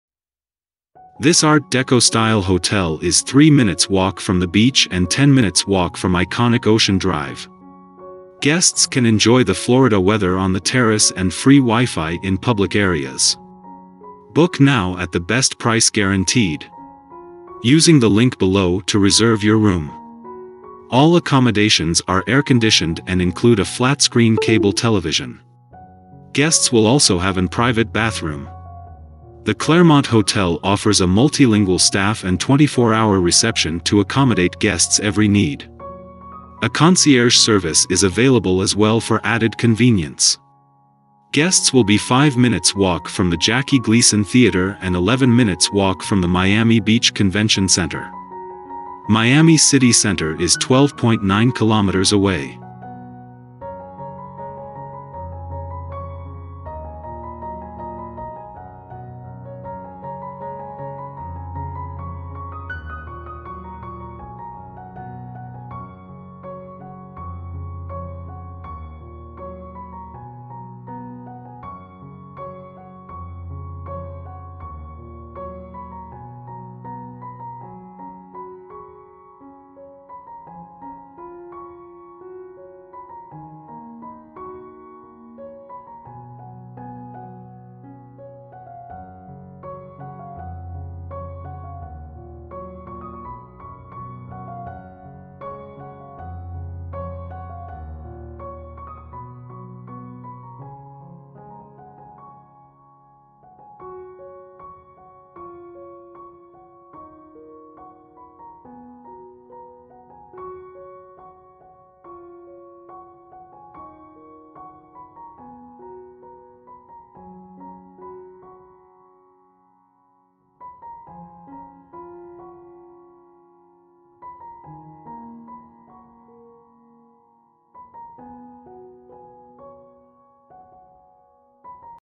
The Claremont Hotel
Book now
This Art Deco-style hotel is 3 minutes’ walk from the beach and 10 minutes’ walk from iconic Ocean Drive. Guests can enjoy the Florida weather on the terrace and free WiFi in public areas.

All accommodations are air-conditioned and include a flat-screen cable television. Guests will also have an private bathroom.

The Claremont Hotel offers a multilingual staff and 24-hour reception to accommodate guests’ every need. A concierge service is available as well for added convenience.

Guests will be 5 minutes’ walk from the Jackie Gleason Theater and 11 minutes’ walk from the Miami Beach Convention Center. Miami city center is 12.9 km away.